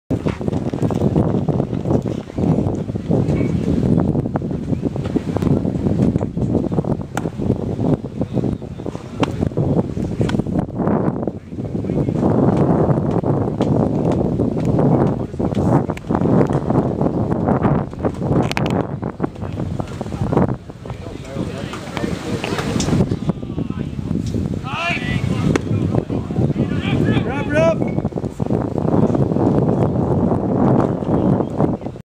Alvie James steals second
Alvie James steals second at the Wod Bat Under Armour National Tournament in West Palm Beach in Florida